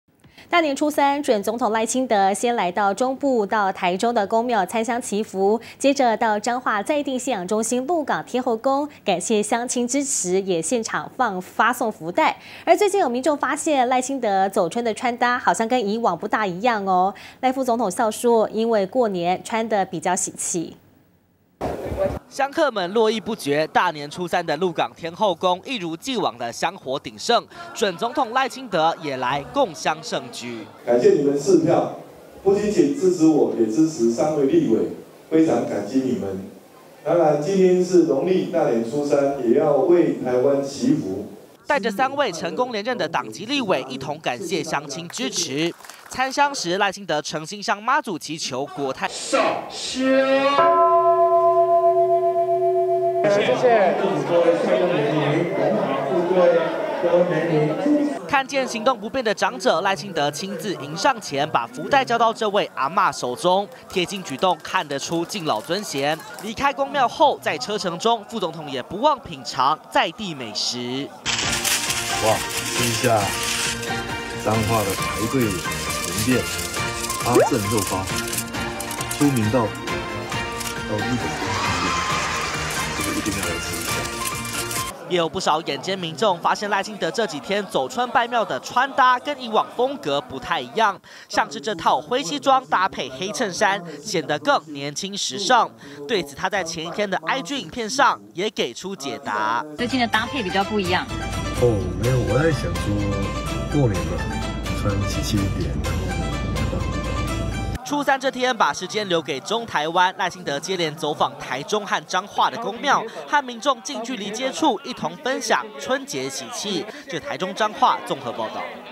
走春造型年輕時尚 賴清德：過年「穿喜氣點」
大年初三準總統賴清德來到中部，先到台中的宮廟參香祈福，接著也來到彰化在地信仰中心鹿港天后宮，不只感謝鄉親的支持，也當場發送福袋。最近有民眾發現，賴清德走春的穿搭，跟以往不太一樣，對此，賴副總統笑說，是因為過年，才穿得比較喜氣一點。